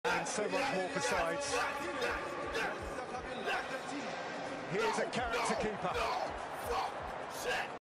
Martinez is a Character Keeper - Peter Drury
Emi Martinez action after receiving the goalkeeper of the tournament award in the FIfa world Cup finals Argentina vs France
Funny